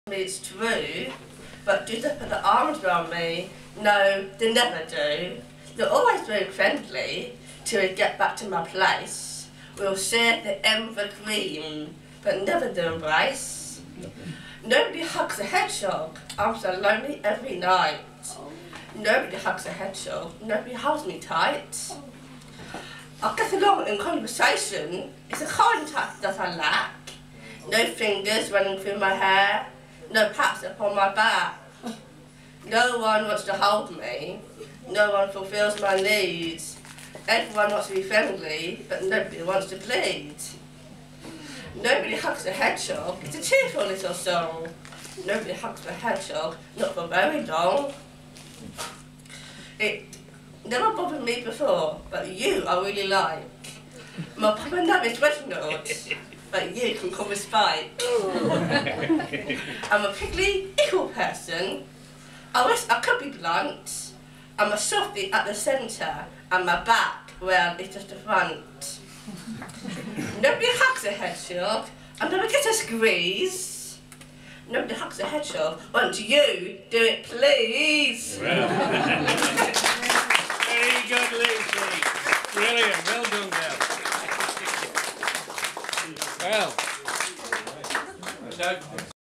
Lucy Grayham Nobody loves a Hedgehog
Here, Lucy Graham reads one of her favourite poems at 'Piggies' Open Mike 28th February 2012.